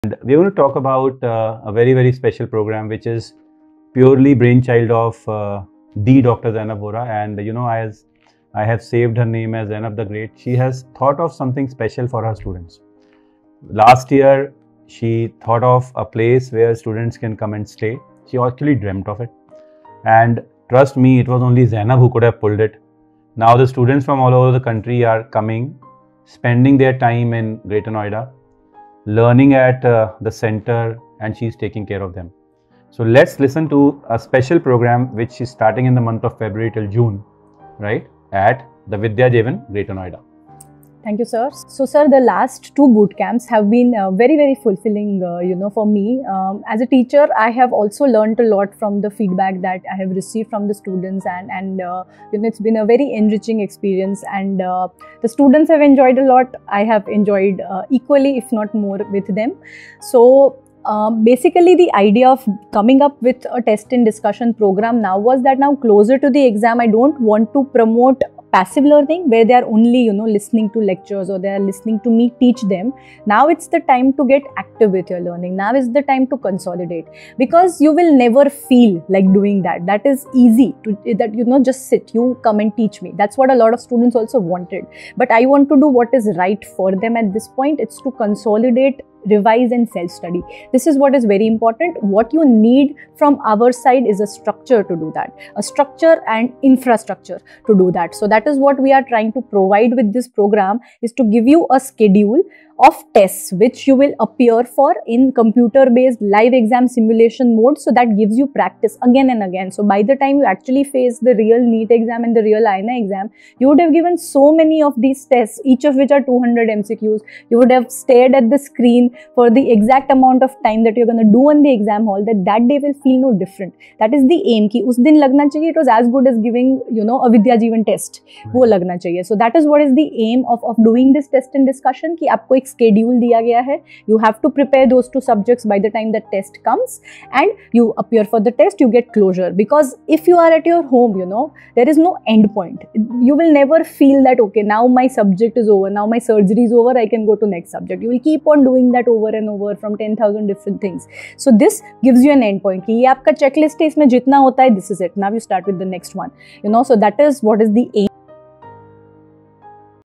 BTR T&D runs Feb–June 2025 with a structured, well-planned schedule.
All You Need to Know About BTR Complete Package at Vidya Jeevan- Dr. Zainab Vora and Dr. Apurv Mehra
In this video, Dr. Apurv Mehra sits with Dr. Zainab Vohra to discuss her vision and the extraordinary program she has brought to life, aimed at revolutionizing the learning journey of medical students across the country.

➡️ The Vision Behind the Initiative
Dr. Zainab Vohra dreamt of a learning space where students could focus solely on their studies, free from distractions.
This dream has been realized in the form of Vidya Jeevan, a center where students from all over India can come to prepare, learn, and grow.

➡️ Timeline and Structure
BTR T&D runs from February to June 2025, providing a structured and well-planned schedule for students.
Tests are held in live computer-based exam simulation mode, designed to replicate real examination conditions.
Students can join on February 16th for an orientation session to get familiar with the place and resources.

From February 17th, they can begin using the facilities. For those interested in specific tests, the first test is scheduled for February 21st.

So, you can directly come on February 21st for the first test, but for the complete package students, there will be an orientation session on February 16th to get acquainted with the facilities.
From February 17th, students can begin using the center actively. It does not start from February 24th; you can start from February 16th itself. All the very best, and we look forward to meeting you on February 16th.

➡️Key Features
Focus on Active Learning and Consolidation
Emphasis on self-study and consolidation rather than passive learning through lectures.
Students receive a structured test schedule with clear deadlines, fostering discipline and accountability.
Each test consists of 200 MCQs, providing targeted practice for NEET PG and INI-CET.

➡️ World-Class Facilities at Vidya Jeevan
Students are provided with personal cubicles equipped with computers for study and test-taking.
The center is open from 7 AM to 9 PM, ensuring a conducive environment for extended study hours.
Limited and safe internet access for doubt resolution and research.

➡️ Simulated Exam Experience
Students practice under exam-like conditions, reducing anxiety and improving performance on the actual test day.
Regular exposure to a structured routine ensures familiarity with test patterns and time management.

➡️ BTR Complete Package Inclusion-
✅ Test Discussions to solidify your concepts.
✅ Dedicated Study Facility with personal cubicles, available from 7 AM to 9 PM, including access to computers for academic purposes.
✅ Personal Guidance & Troubleshooting to address your challenges effectively.
✅ Expert Mentorship by Dr. Apurv Mehra and Dr. Ravi Sharma for valuable insights and strategies.
✅ Daily Custom Modules on CBT to keep you accountable and track your progress.
✅ 4-Day BTR Offline Session: 14th–17th April for an intense boost.
✅ 4-Day GT Program: 30th May–2nd June to ace your preparation.

➡️ PYQs and Test Practice:
Focus on past year questions (PYQs) for NEET, INI-CET, and FMGE, covering at least the last three years.

➡️ Weakness Targeting:
Identify weak areas using test results and BTR as a diagnostic tool.

➡️ Supplementary Learning:
Use main videos, YouTube content, and other resources to clear doubts.

➡️ Holistic Environment
A supportive atmosphere where students help and motivate one another.
A blend of individual and group study spaces, including gardens, auditoriums, and silent zones.

➡️ Why Vidya Jeevan Stands Out

Simulation for Success:
Like pilots practice in flight simulators, students practice under test-like conditions, ensuring preparedness.

Structured Routine:
The program eliminates procrastination by providing a clear checklist and end-point goals.

Motivational Ecosystem:
Being surrounded by peers with similar goals fosters a sense of camaraderie and competition.

Support Beyond Academics:
Small but significant details like meals, fruits, and a stress-free environment ensure students can focus entirely on their studies.

This program, a collaboration of visionaries like Dr. Zainab Vohra and Dr. Ravi Sharma, is designed to transform the preparation experience into a journey of growth, learning, and success.

The goal is not just to prepare students for exams but to help them find joy in learning while providing the infrastructure and support they need to excel.